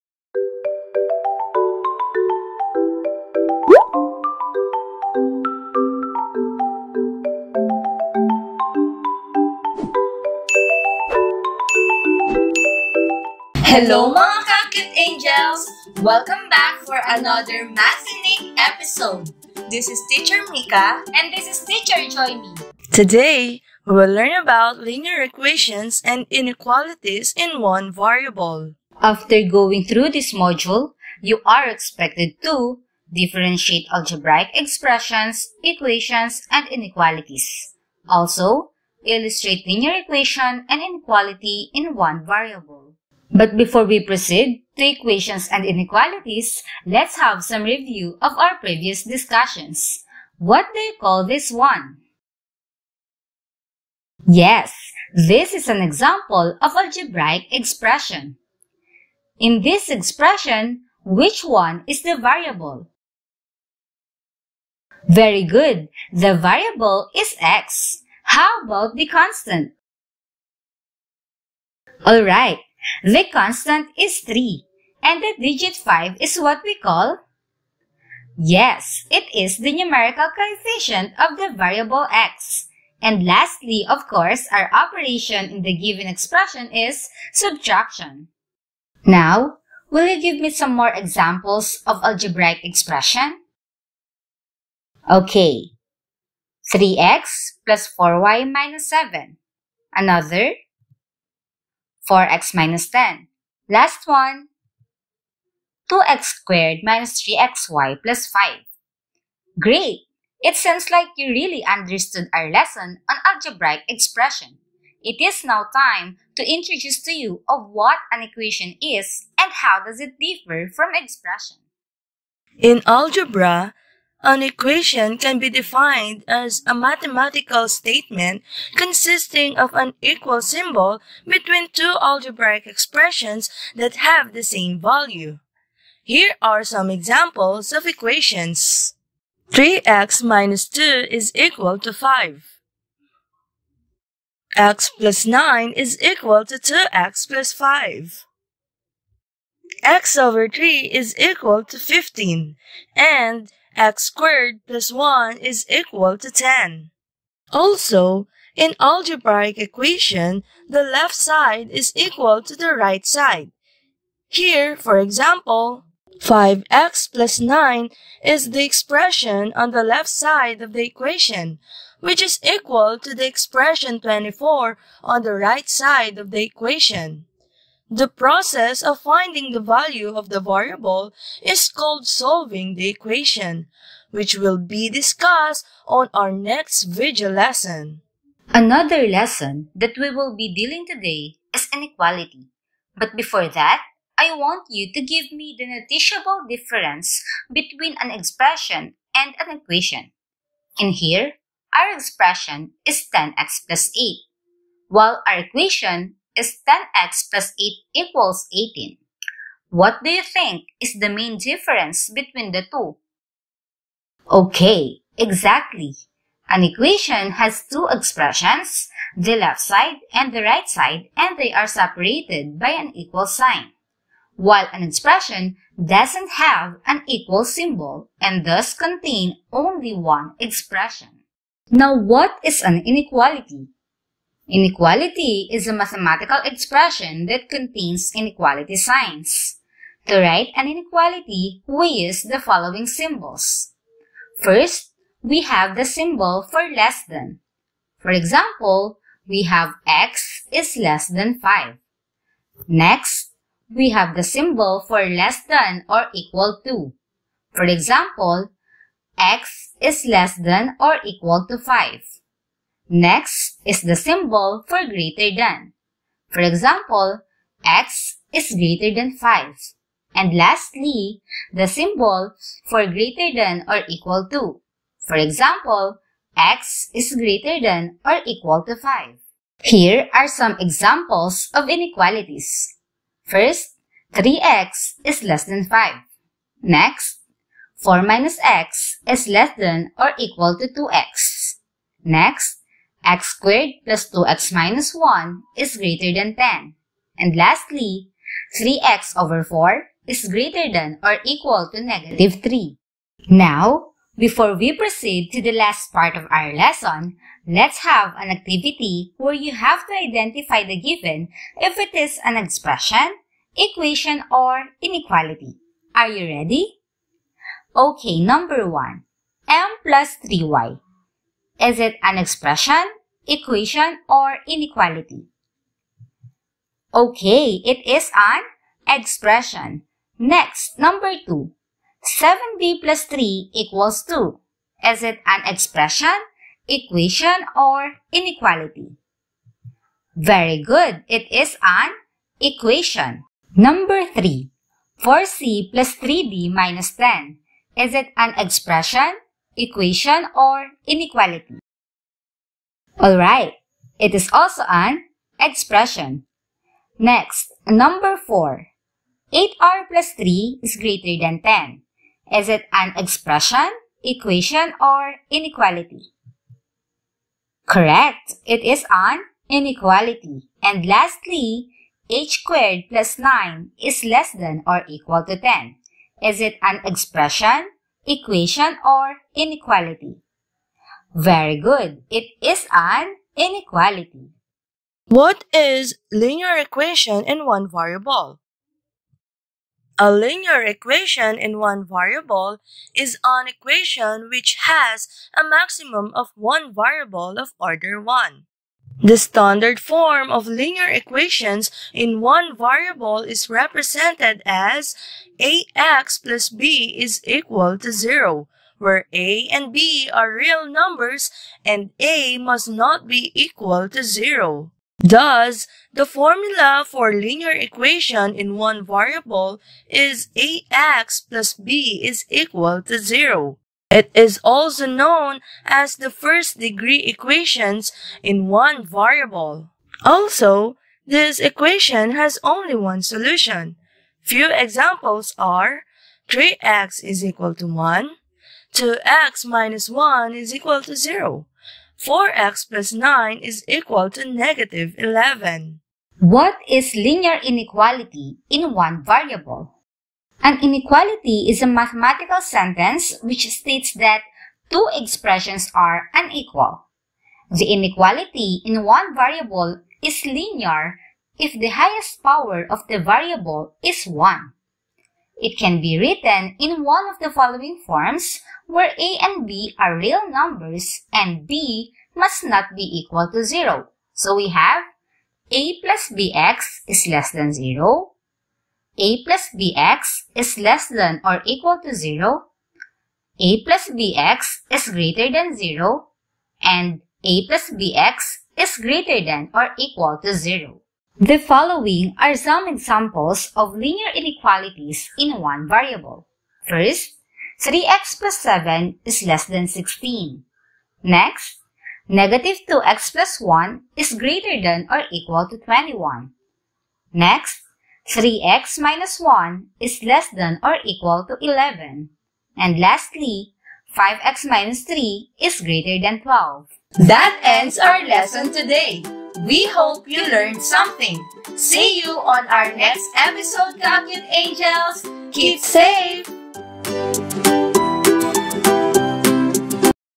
Math 7 ll Linear Equations and Inequalities In One Variable l Acute Angels
Hello mga Ka-@AcuteAngels ! In this video, you will learn about Worded Problems Involving Algebraic Expressions. Enjoy!

Reference: Compendium of Notes In Mathematics
(MELC Based) Schools Division of Tarlac Province